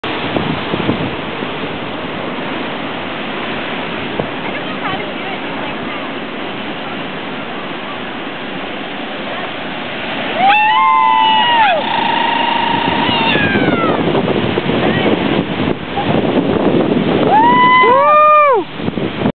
tubed in honalua bay
my long honalua wave, I get 2 kind of short barrels and a few manuevers in